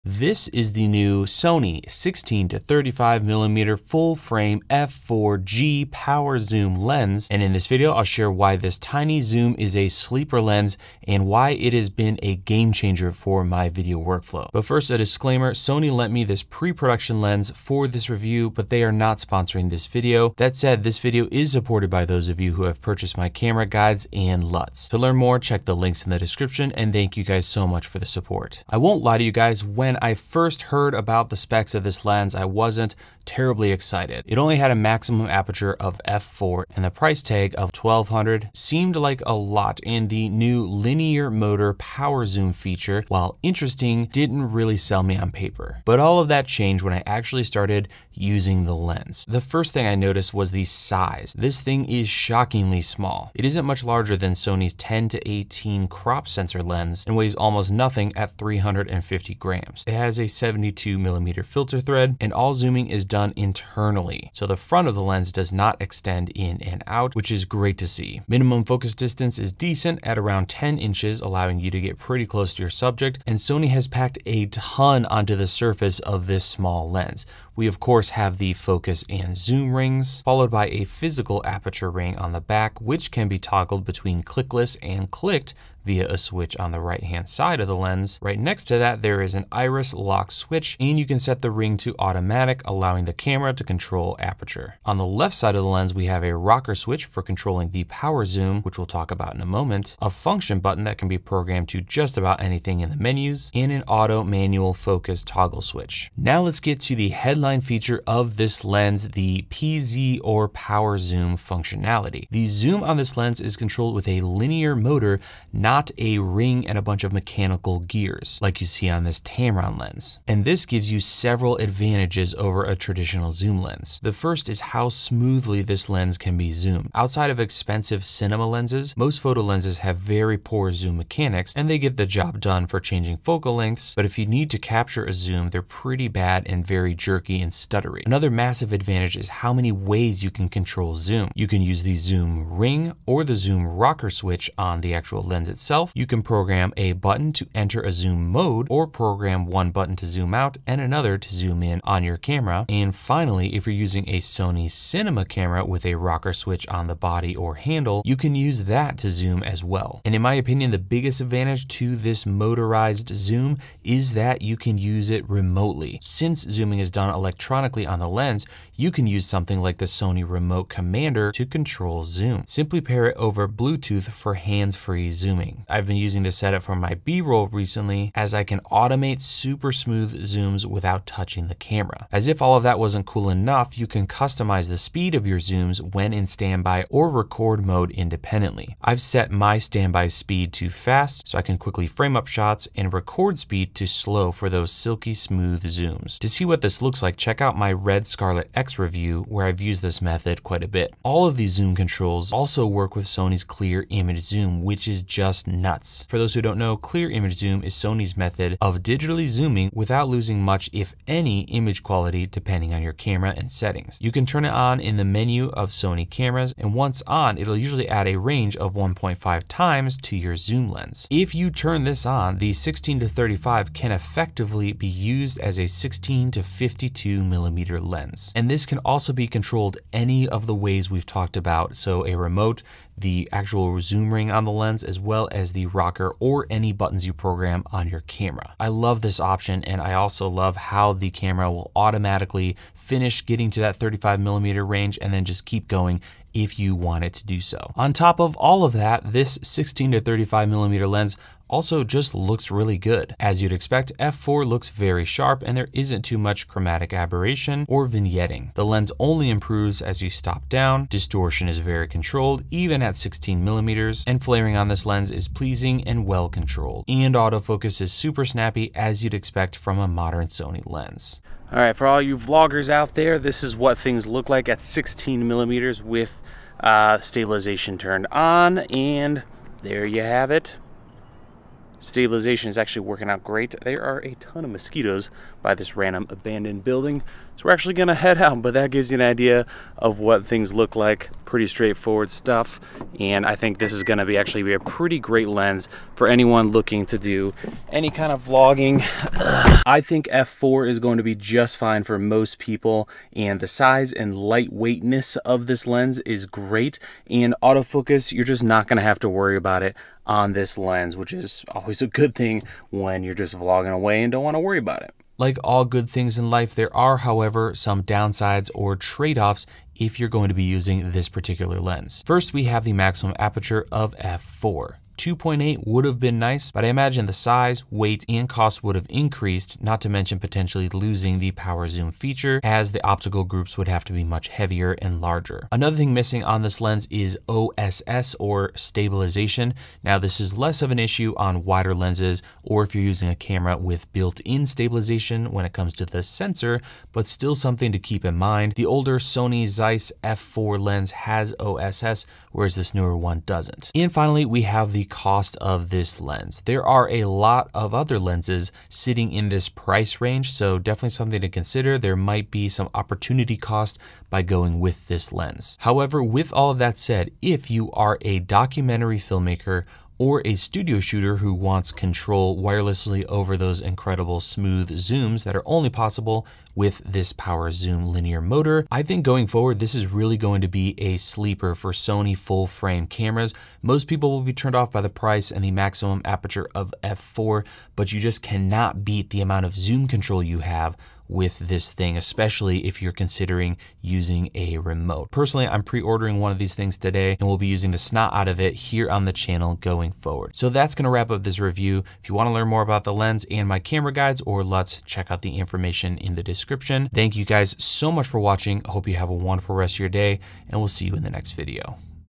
This Sony Power Zoom Lens ROCKS! Sony 16-35mm f/4 PZ Review
Review of the Sony FE PZ 16-35mm f/4 G lens. See why this full frame wide angle zoom is so good for video production.

Sony FE PZ 16-35mm f/4 G
🛒 on B&H (PAID LINK)
🛒 on Amazon (PAID LINK)

VIDEO CHAPTERS
0:00 Introduction
0:28 Lens overview
1:45 Power Zoom Functionality
2:39 Remote Control and Settings
3:20 Clear Image Zoom
4:07 Image Quality and Footage
5:14 Downsides and Tradeoffs
6:05 Final Thoughts and Recommendations

PROS & CONS
+ Full frame
+ Smooth linear zoom motor
+ Fast auto focus
+ Physical aperture ring with de-clicking switch
+ Zoom ring and rocker switch
+ Small and lightweight
+ Can be remotely controlled
- F4 maximum aperture
- No O.S.S. (Optical steady shot)

Sony loaned me a pre-production 16-35mm for this review.